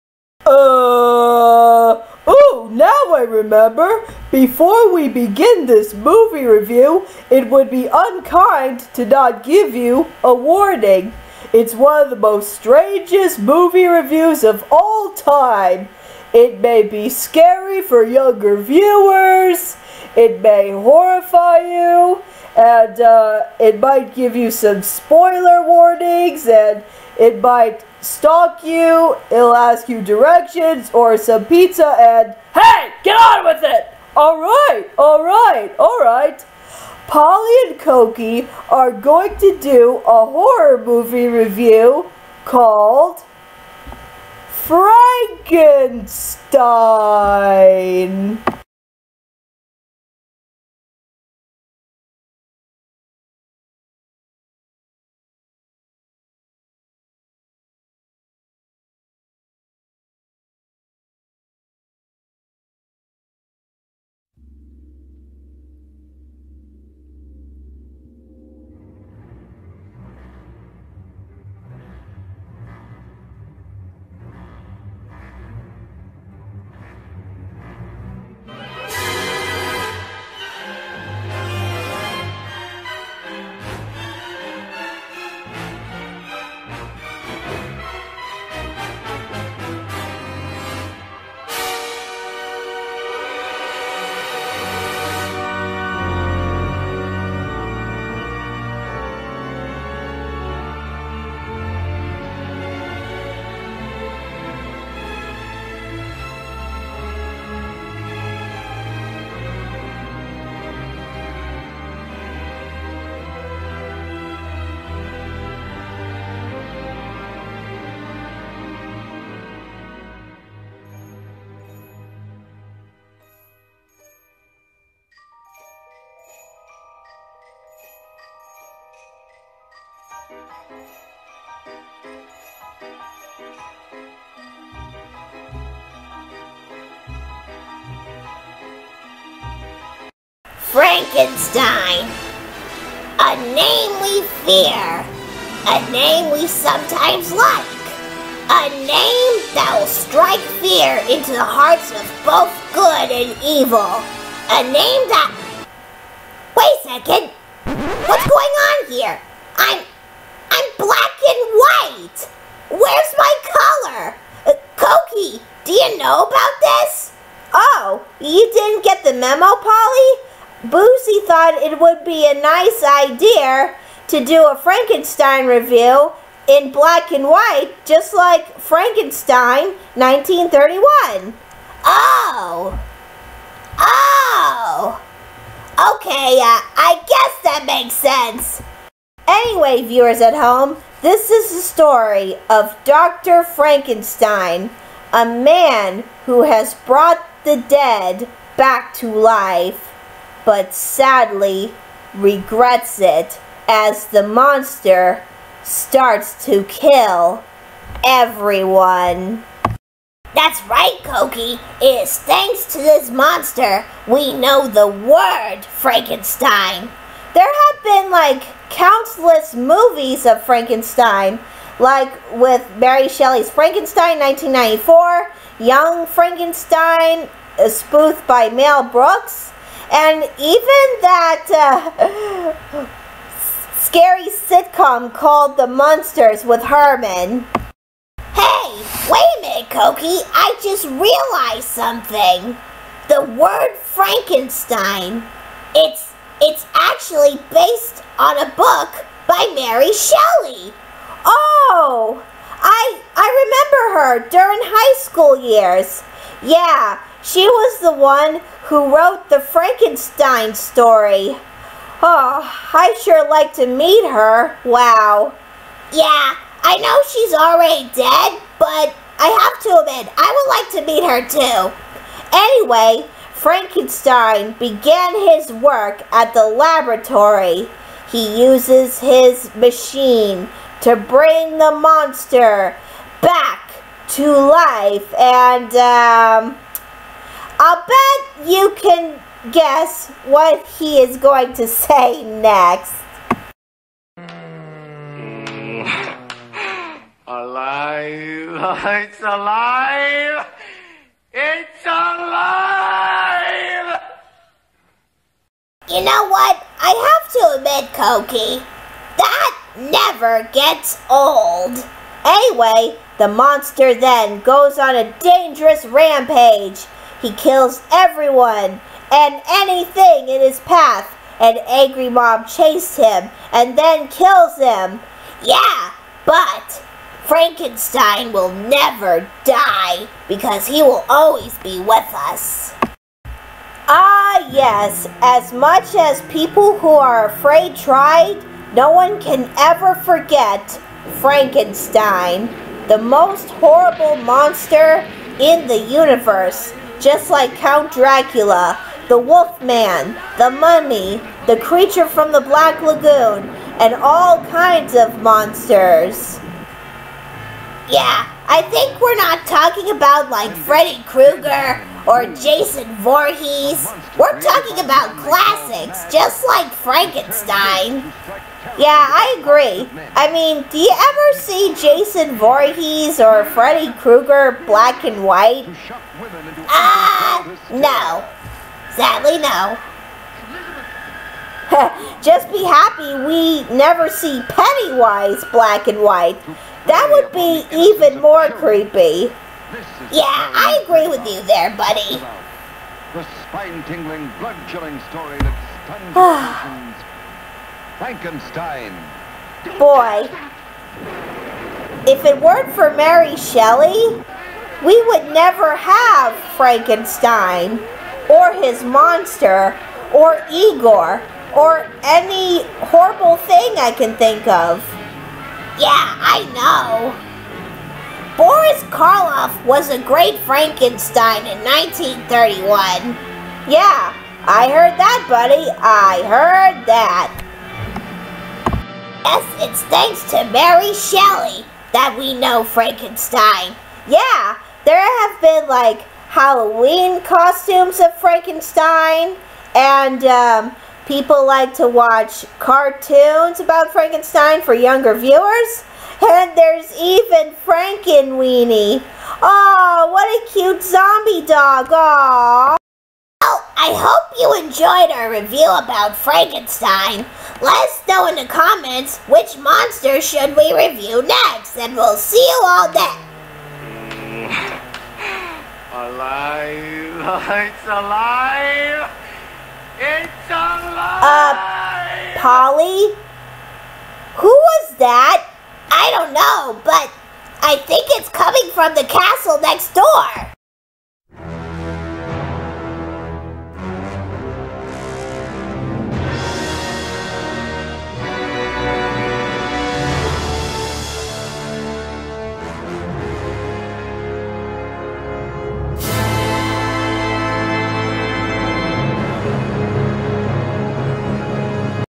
Polly and Koki Review Frankenstein
Polly and Koki made a review about Frankenstein but Polly made a shocking discovery. Polly and Koki are black and white!